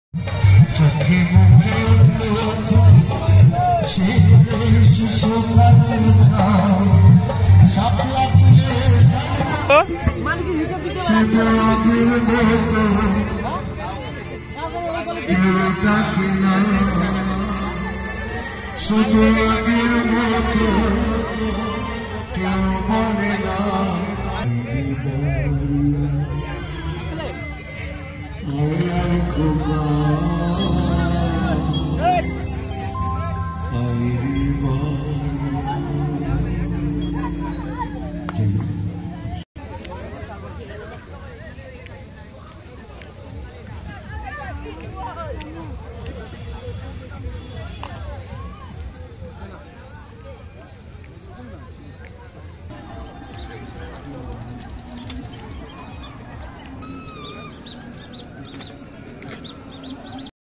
ghugni: Kolkata's famous street food.
this hardworking man selling ghugni for his family from morning to evening.yummy food of kolkata.kolkata street food